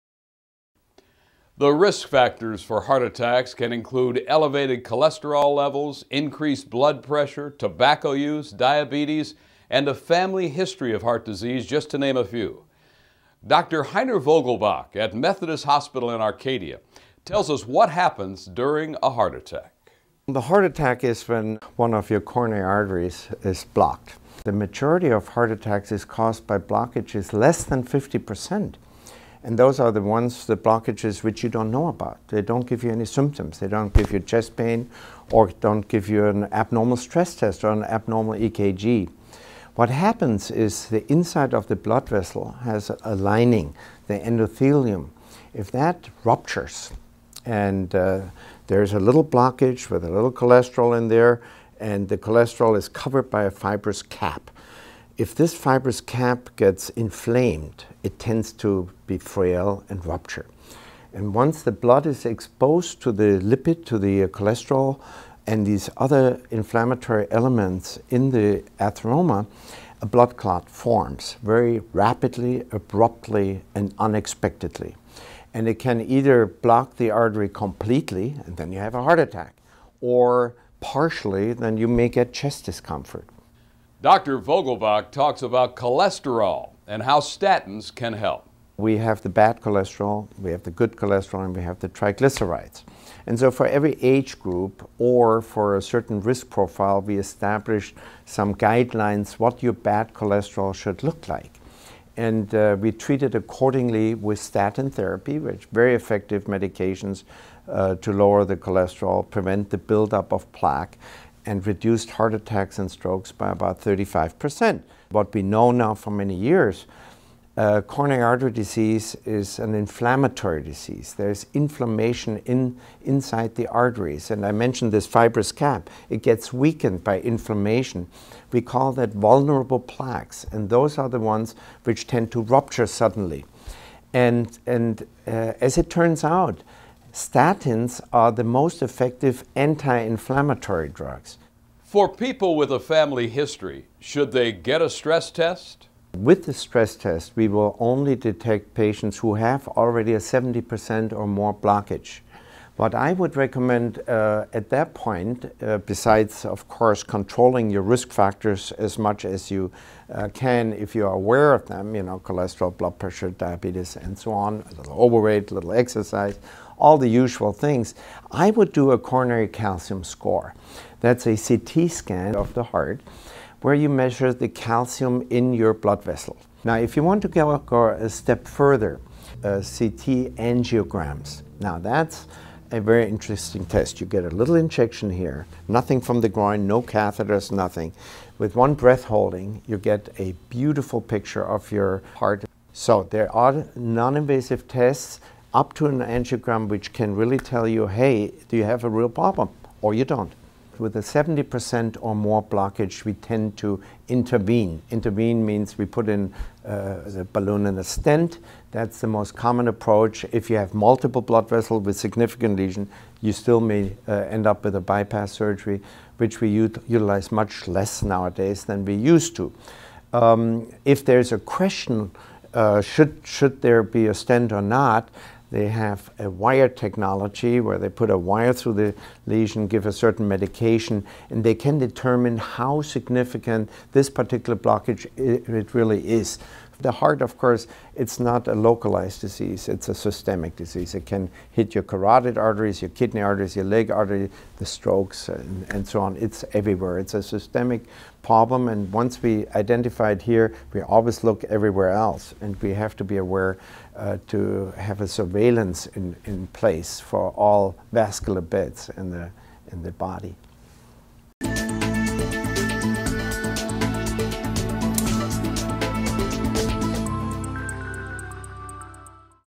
Statin Therapy To Prevent Heart Attacks - Heiner Vogelbach, MD
The risk factors for heart attacks can include elevated cholesterol levels, increased blood pressure, tobacco use, diabetes, and a family history of heart disease just to name a few. Doctor Heiner Vogelbach at Methodist Hospital in Arcadia, tells us what happens during a heart attack.